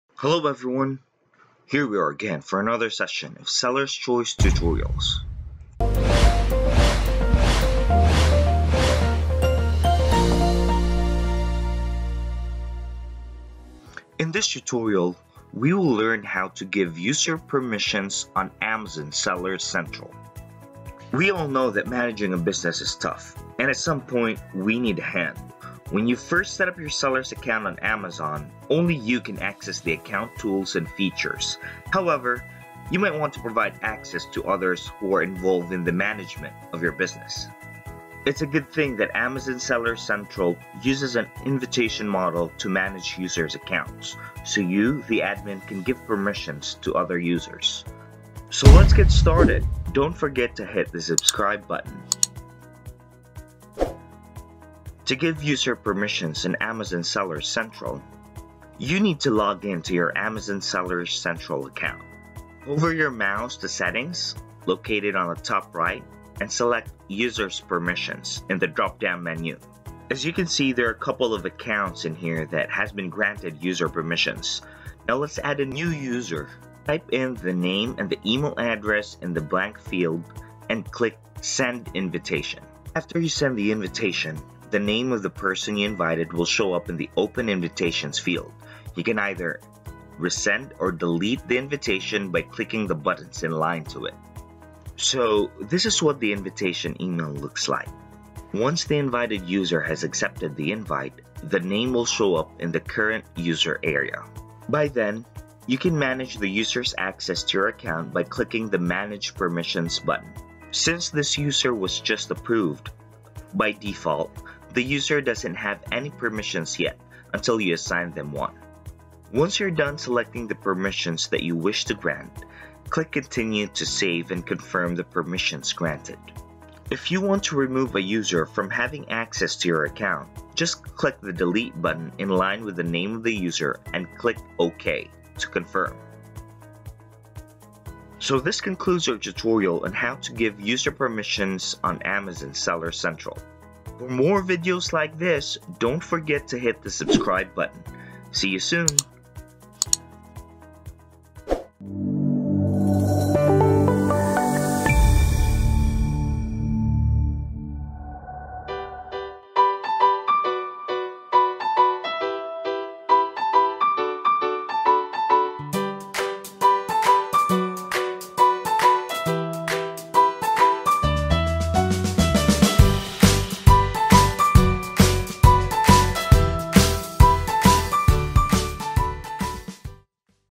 How to Give User Permissions on Amazon Seller Central - E-commerce Tutorials
Today, we're showing you how to give user permissions on Amazon Seller Central so you can grant somebody a access to your account.